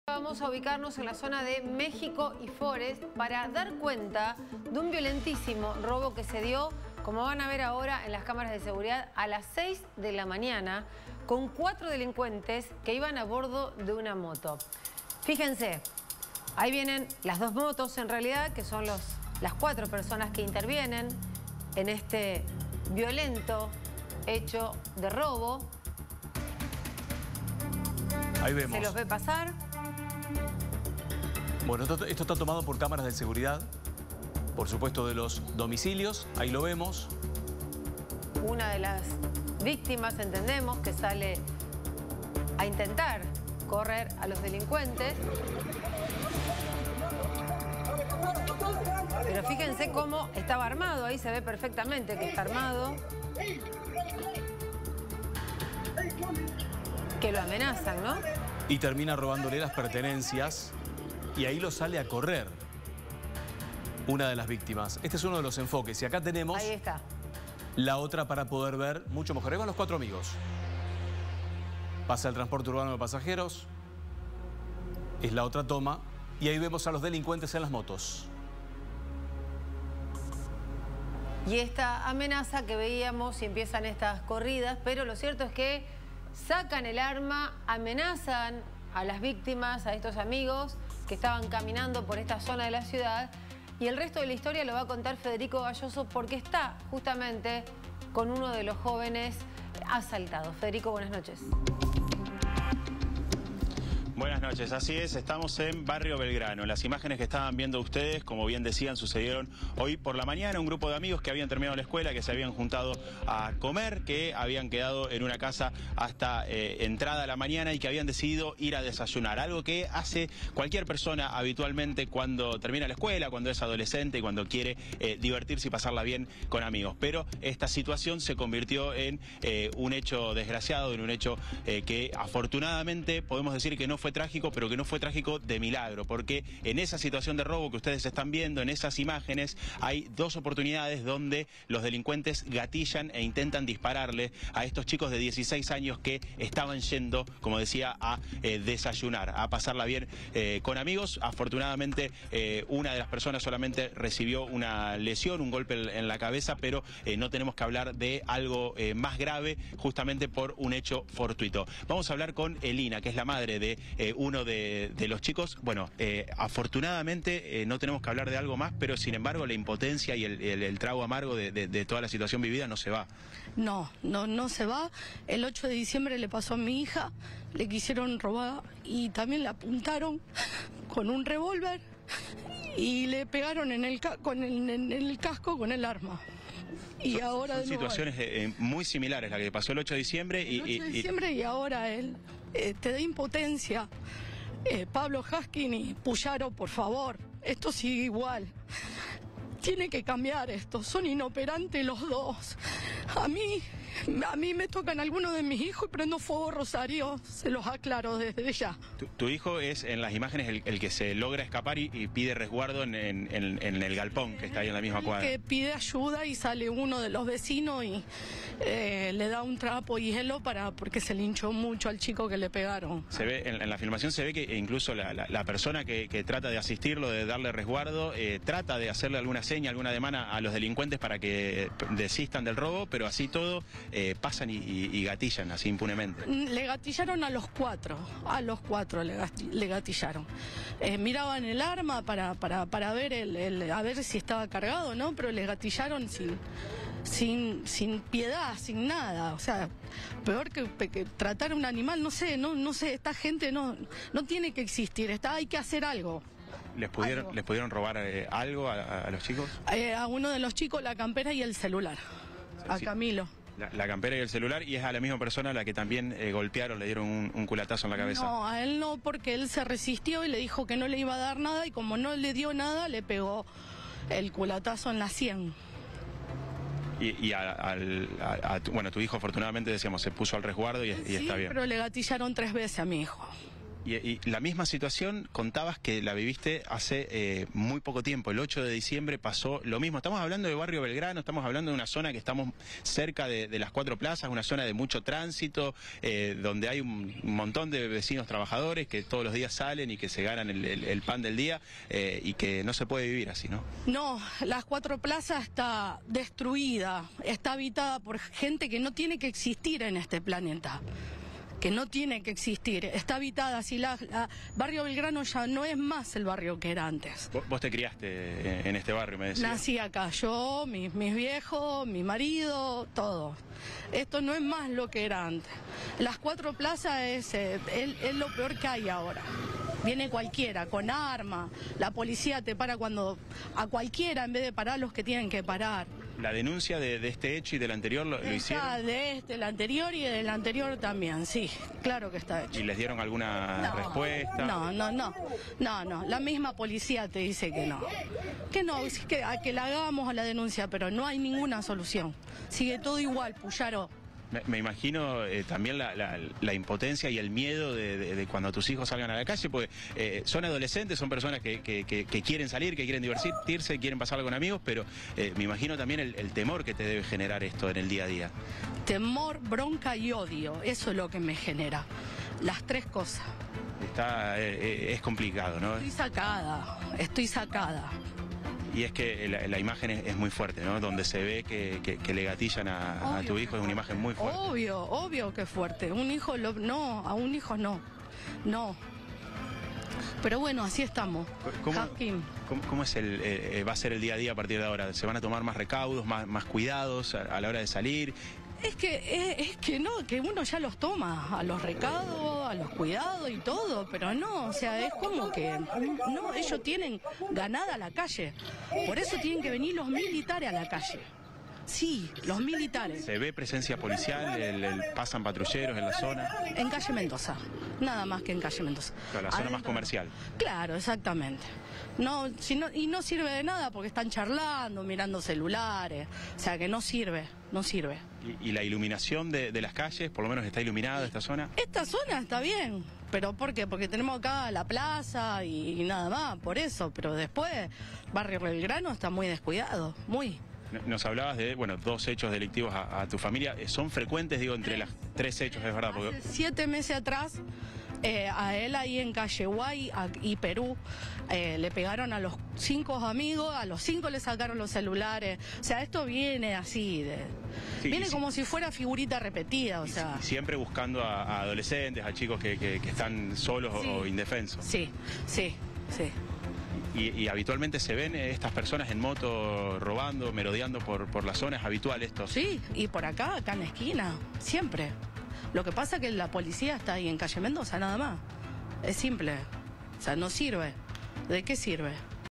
Motochorros armados asaltaron violentamente a un grupo de adolescentes
Las víctimas se habían reunido y fueron asaltadas en la calle cuando iban a desayunar.